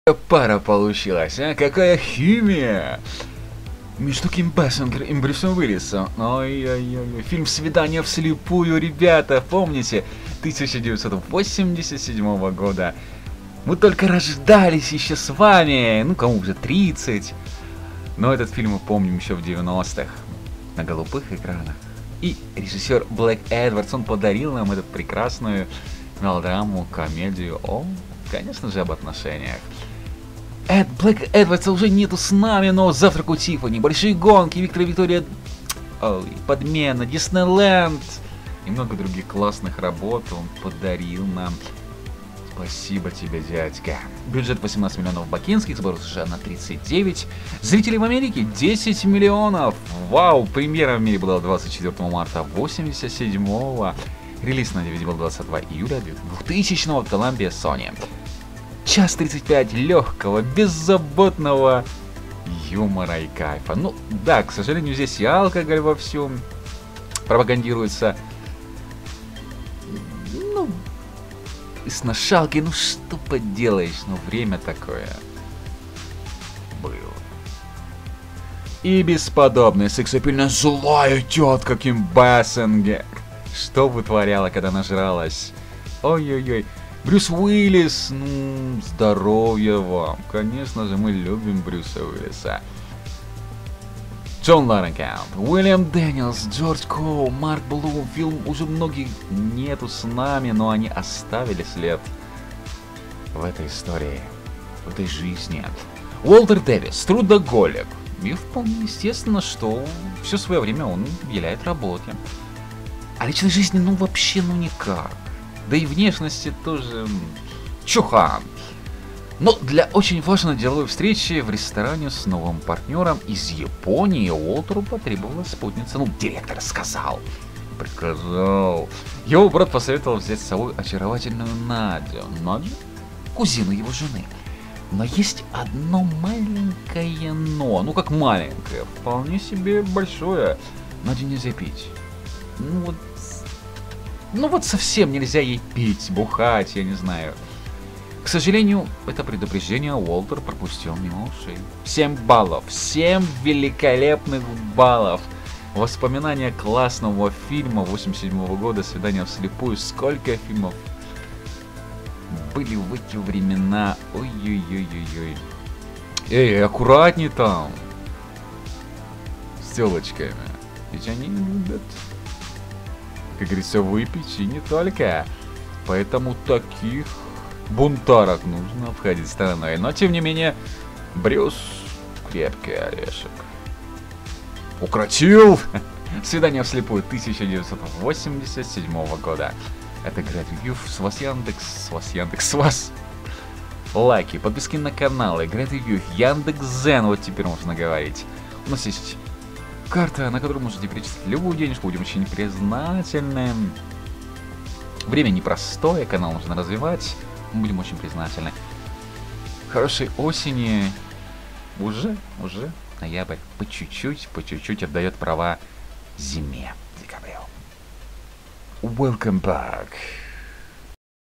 Blind Date (1987) ➤ Review (GR)
Walter Davis is a workaholic, and it's only natural that he pays all his attention to work, rather than his personal life or appearance. For a very important business meeting in a restaurant with a new partner from Japan, Walter needed a companion.

His brother advised him to take the charming Nadia, his wife's cousin, with him. But there is one thing: Nadia cannot drink... Absolutely! Unfortunately, Walter ignored this warning.

PSB Bank - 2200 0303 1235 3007